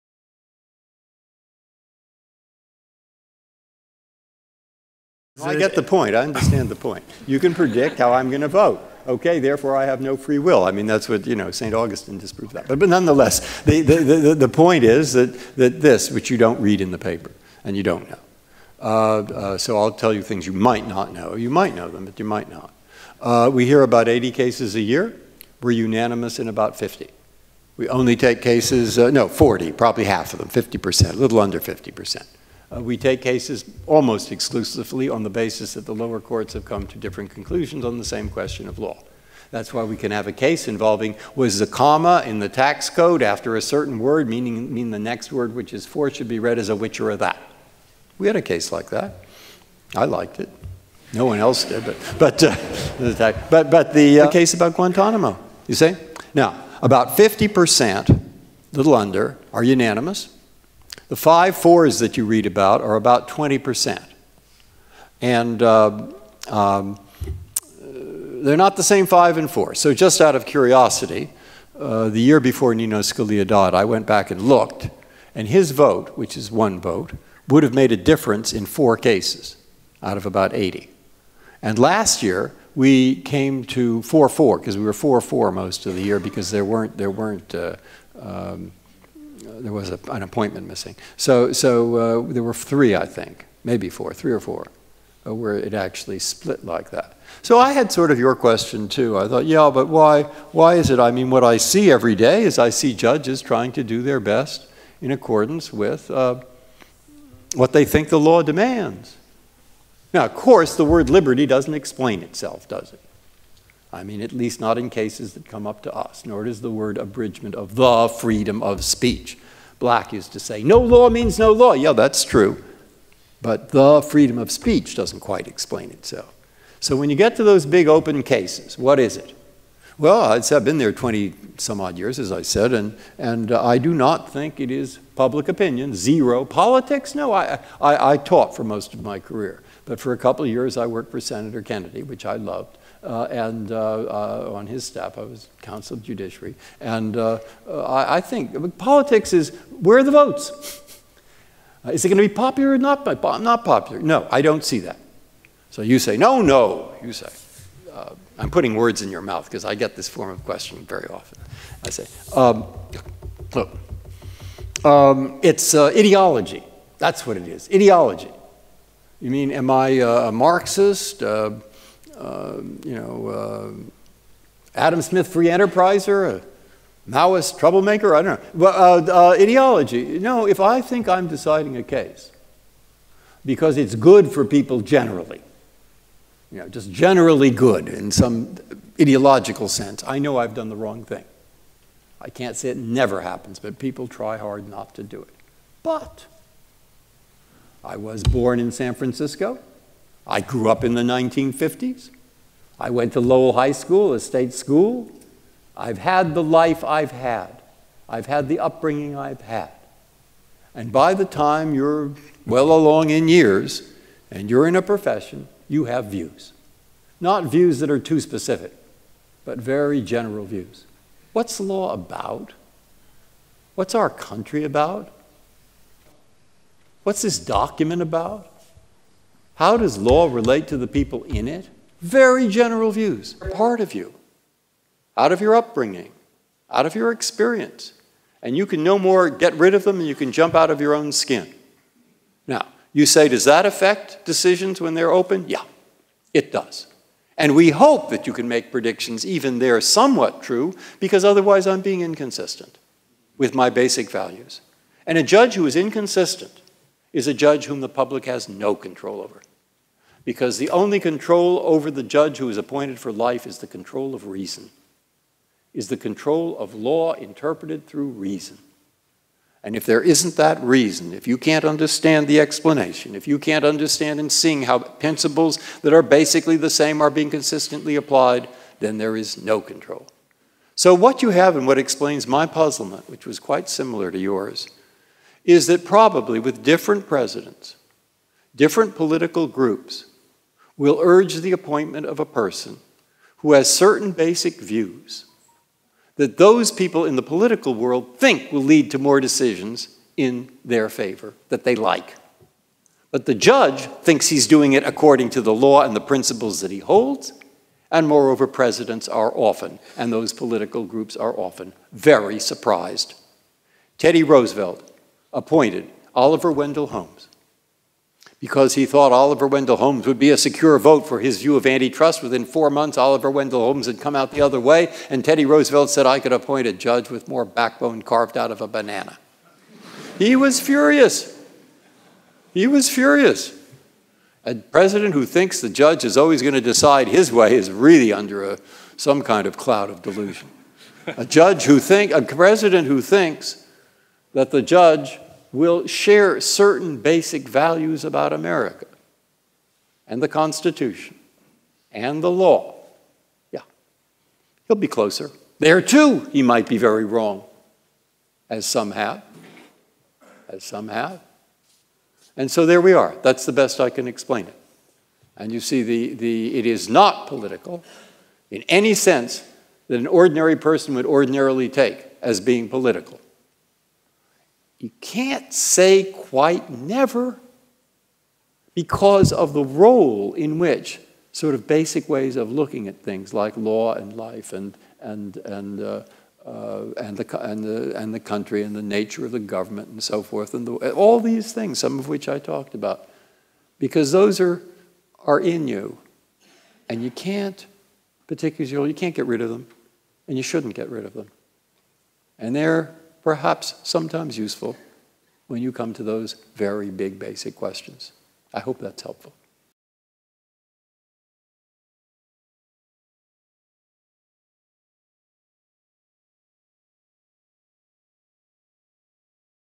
Justice Breyer: are Supreme Court appointments political?
Nexus Lecture 2017 'An Apology for the Law' by Supreme Court Justice Stephen Breyer